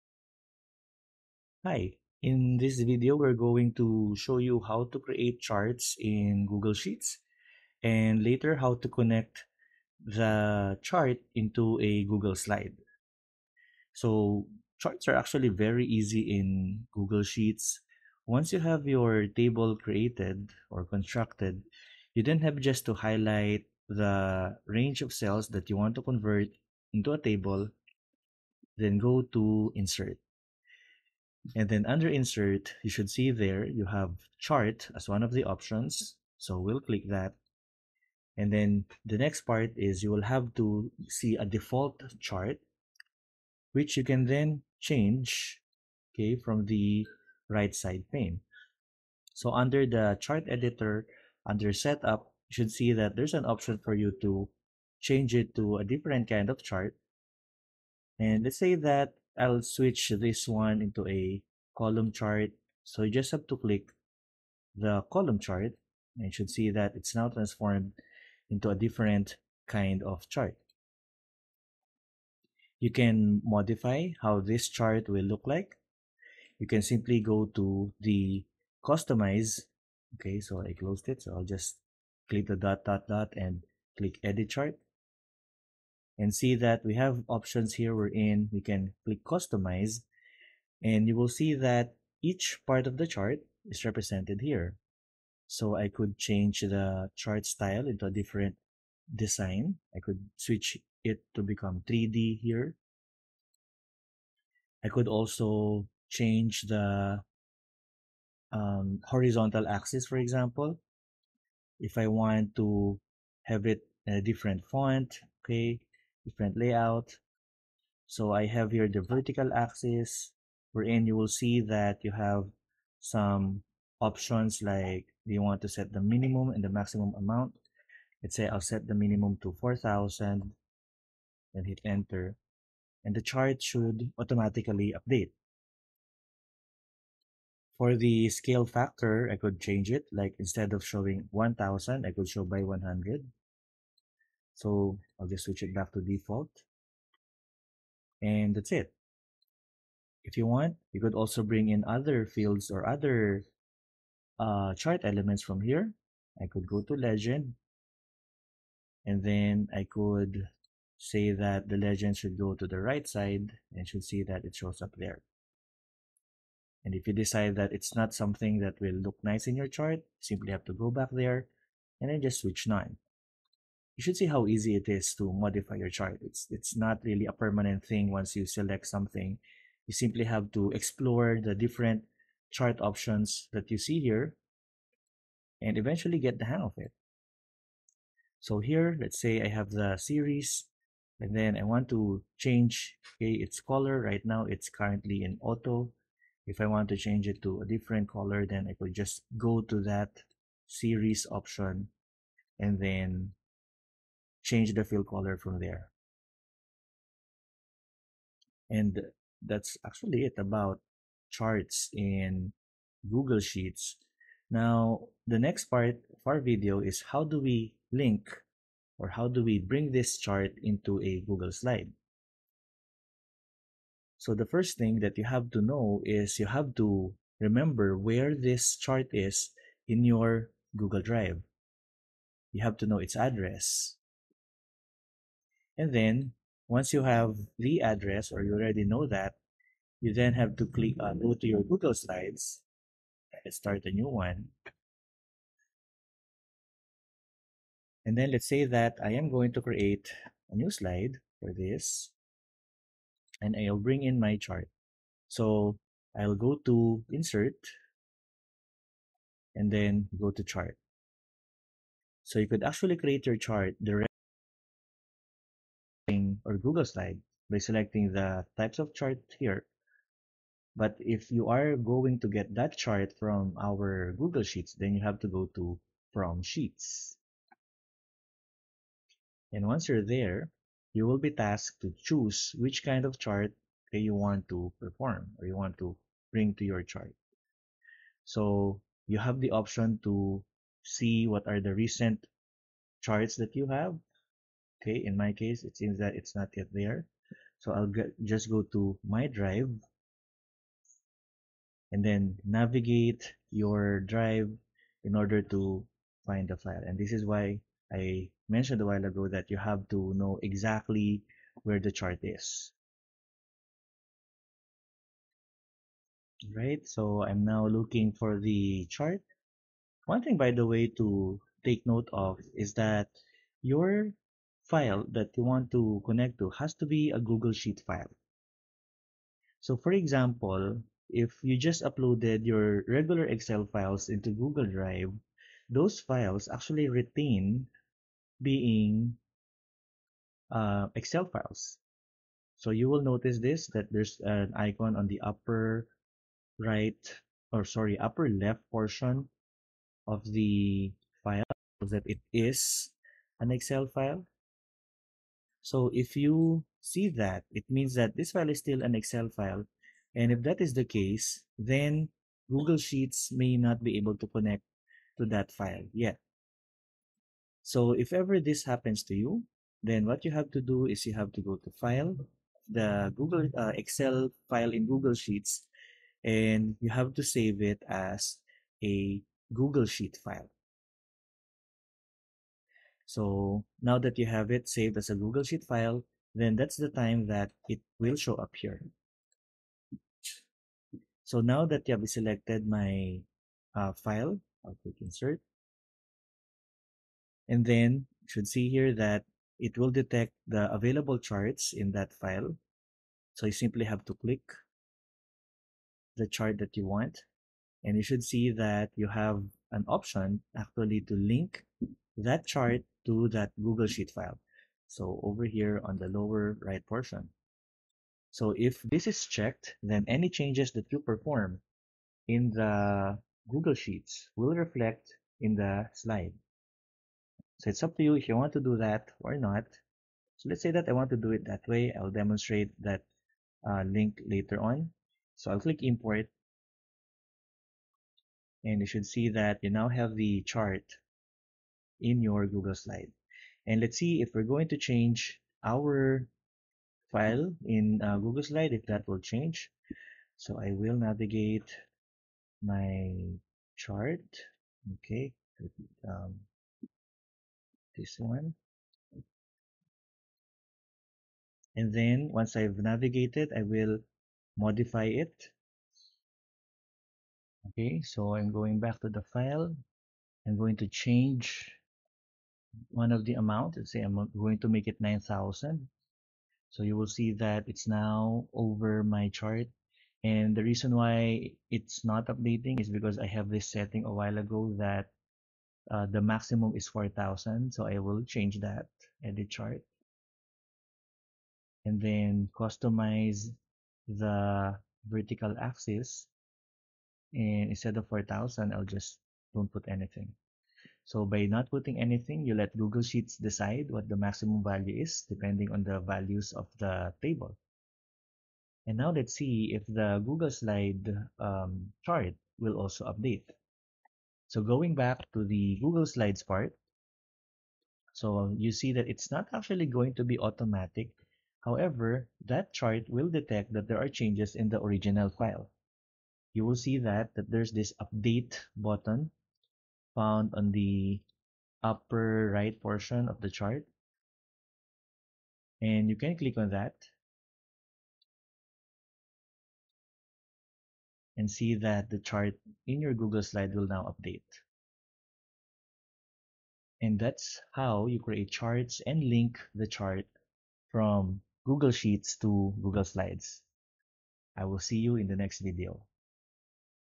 Create and Link Charts from Google Sheets to Google Slides
This video shows how to create charts in Google Sheets, modify chart elements, and link the chart to Google Sheets.  It will also explain what to do in case the chart or sheet does not show up when you are attempting to connect the chart in Google Slides.